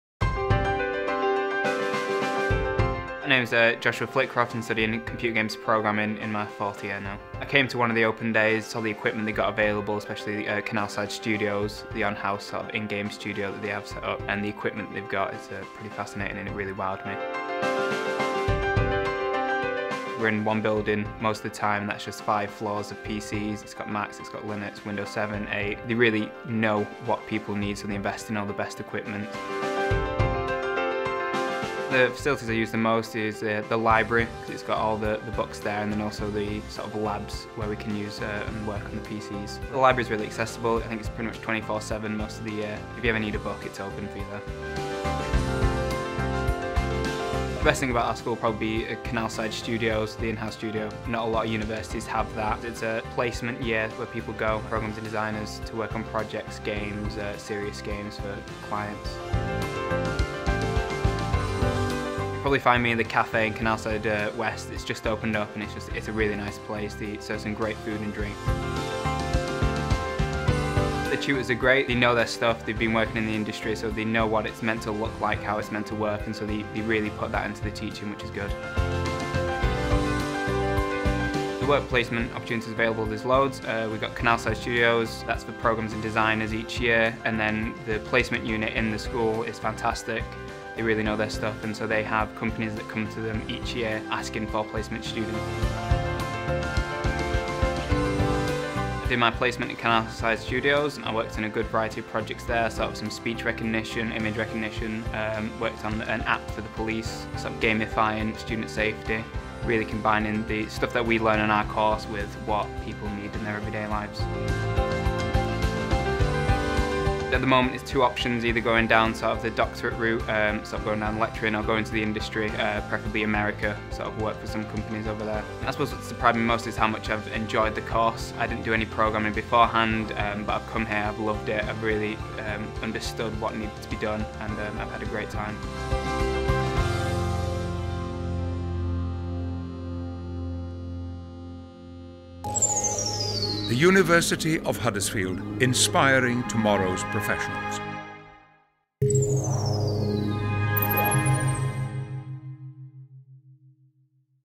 Computing at the University of Huddersfield - Joshua Flitcroft
Joshua is a 4th year student of a Computing Games Programming BSc(Hons) degree. He chose Huddersfield because of the facilities available such as the game studio which he saw at an Open Day. Joshua undertook a work placement at Canalside Studios based at the University and worked on a variety of projects including speed recognition and image recognition using knowledge he learned on his course. He wants to work in America or study for a doctorate and become a lecturer.